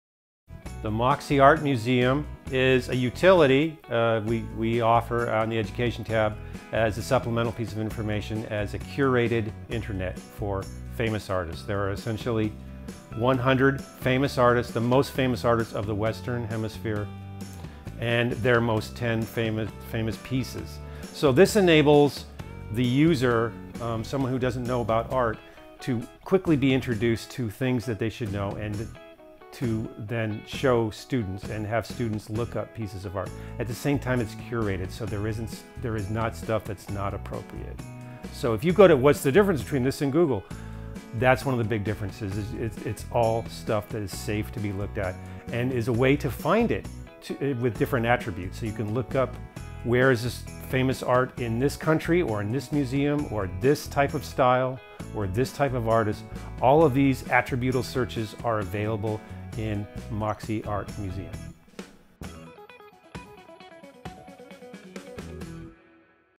Matt Kirby, Creator of Apples To Apples, Explains the On Line Resourse, MoxieBox Museum!
Need to look up a famous artist, painting or sculpture? MoxieBox Museum is a free resource available for all to use! You are one click away from discovering something fascinating about a famous artist, location or piece of art! MoxieBoxArt.com